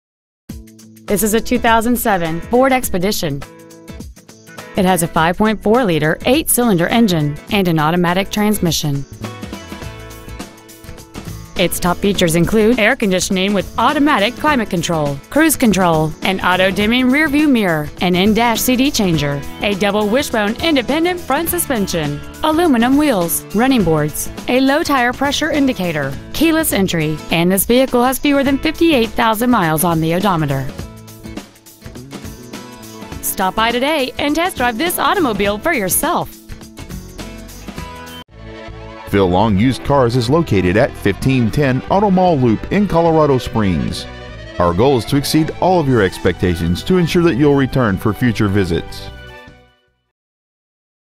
2007 Ford Expedition EL Denver CO
We are proud to present this 2007 Ford Expedition EL .

 Year : 2007

 Make : Ford

 Model : Expedition EL

 Engine : 5.4L SOHC SEFI 24-valve V8 engine

 Trans . : Automatic

 Exterior : Blue

 Miles : 57,121

 1510 Auto Mall Loop

 Used 2007 Ford Expedition EL Colorado Springs CO